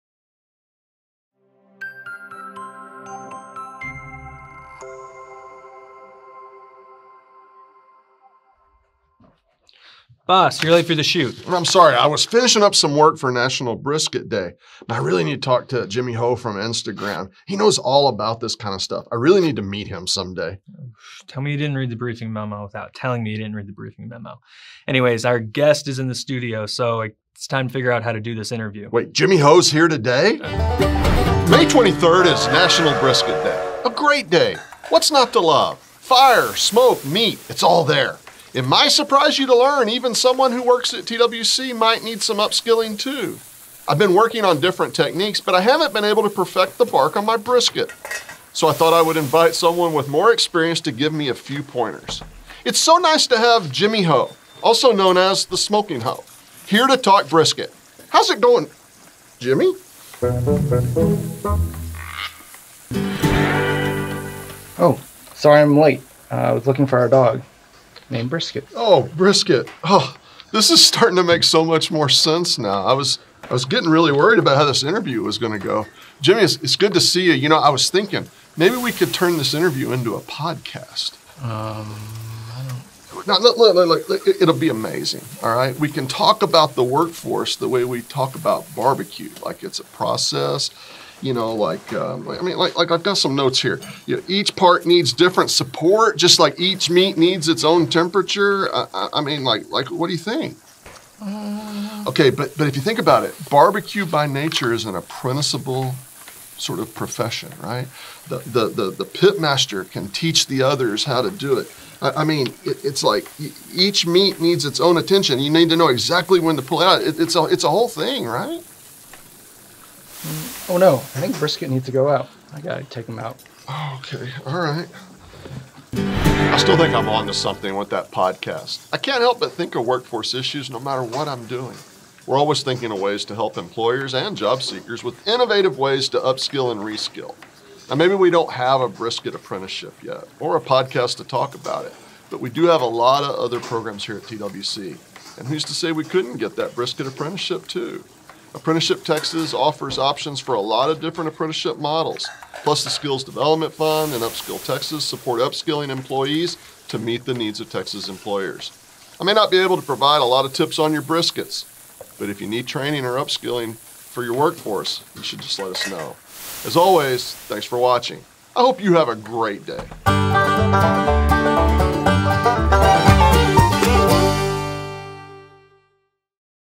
National Brisket Day | Chairman Bryan Daniel VLOG | May 23, 2024
TWC Chairman Bryan Daniel is getting ready for National Brisket Day by spending the day in the kitchen with Jimmy Ho, one of Texas' well-known BBQ connoisseurs. But it's their co-star, Brisket, who absolutely stole the show!